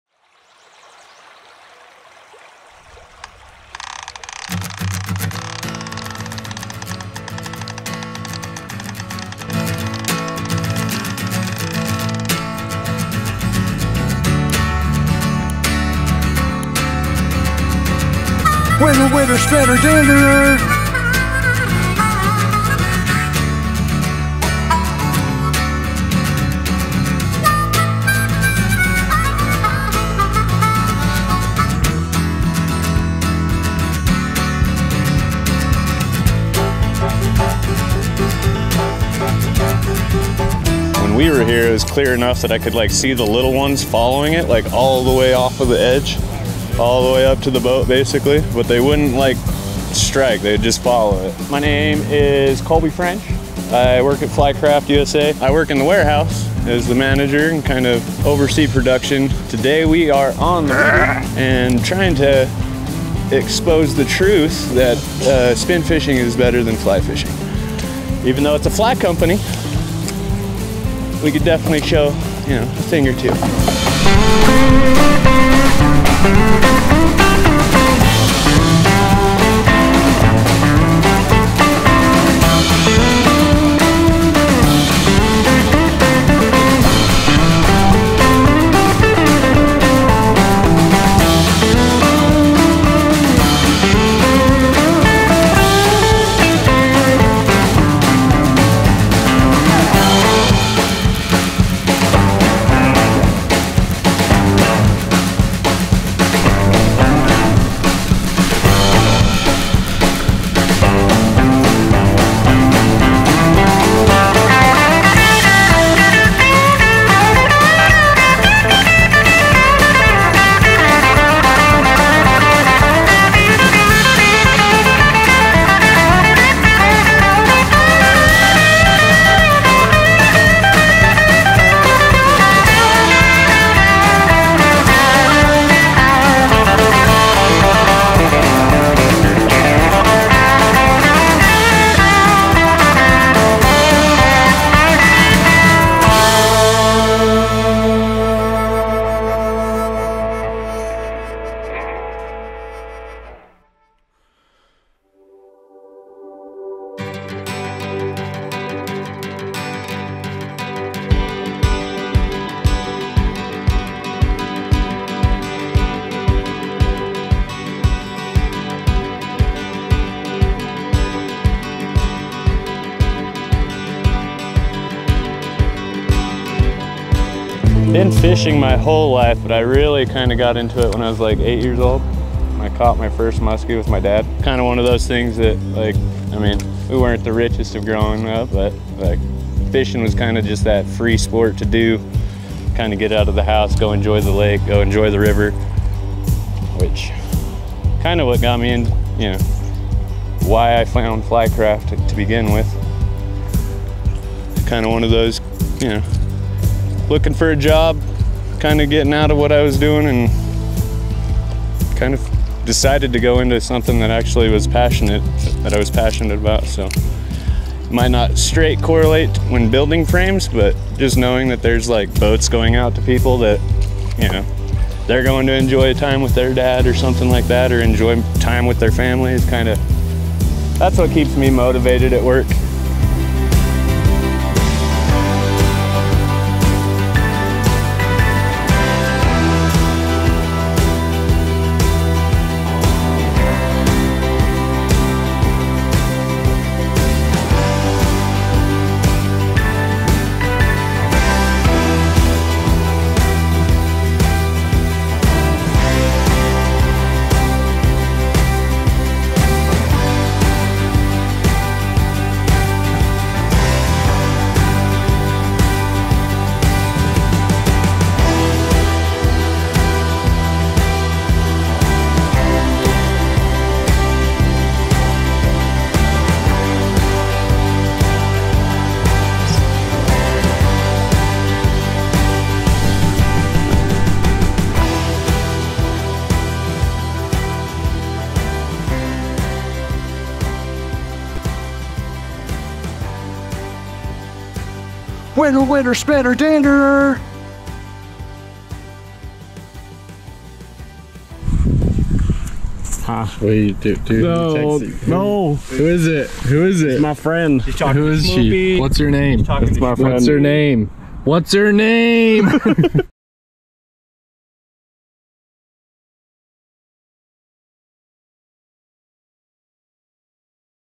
Spin Fishing From A FLYCRAFT | With Colby French
Join us for a day with our Flycraft Production Manager, Colby French, in our first spin fishing video that we have put out. As much as we like to fly fish, we realized that conventional fishing needs its place in the spotlight too, because let's face it, it's super fun! Whether fly fishing or throwing spinners or lures the Flycraft makes it happen.

Featured in this video: Flycraft 3-Man Guide Model